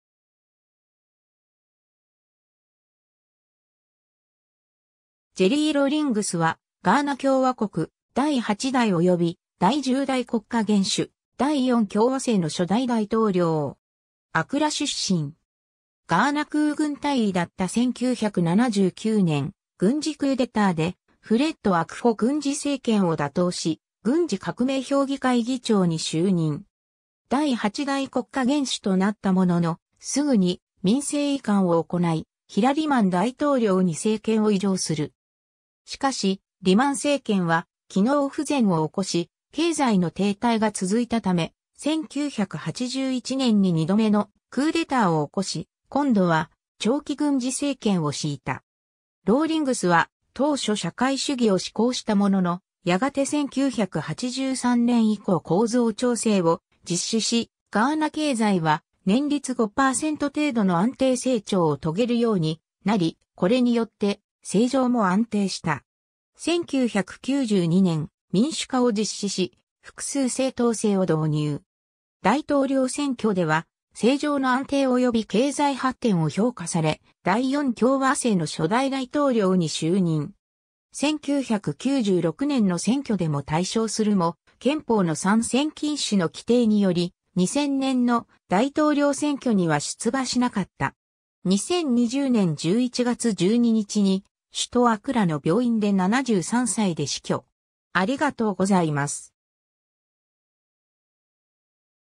ジェリー・ローリングス
インターアクション・カウンシルの人物
ジェリー・ローリングス（英語: Jerry Rawlings、1947年6月22日 - 2020年11月12日）は、ガーナ共和国第8代および第10代国家元首、第4共和政の初代大統領。
アクラ出身。
 ガーナ空軍大尉だった1979年、軍事クーデターでフレッド・アクフォ軍事政権を打倒し、軍事革命評議会(AFRC)議長に就任。
第8代国家元首となったもののすぐに民政移管をおこない、ヒラ・リマン大統領に政権を移譲する。
しかし、リマン政権は機能不全を起こし、経済の停滞が続いたため、1981年に2度目のクーデターを起こし、今度は長期軍事政権を布いた。
 ローリングスは、当初社会主義を志向したものの、やがて1983年以降構造調整を実施し、ガーナ経済は年率5%程度の安定成長を遂げるようになり、これによって政情も安定した。
 1992年、民主化を実施し、複数政党制を導入。
大統領選挙では政情の安定及び経済発展を評価され、第4共和政の初代大統領に就任。
1996年の選挙でも大勝するも、憲法の三選禁止の規定により、2000年の大統領選挙には出馬しなかった。
 2020年11月12日に首都アクラの病院で73歳で死去。